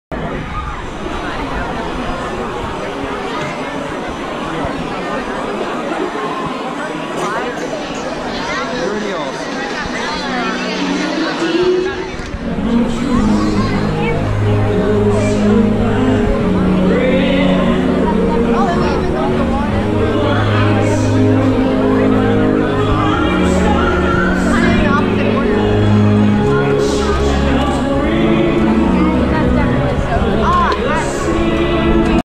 Main Street Snow
The snow is falling before the parade at Magic Kingdom during A Very Merry Christmas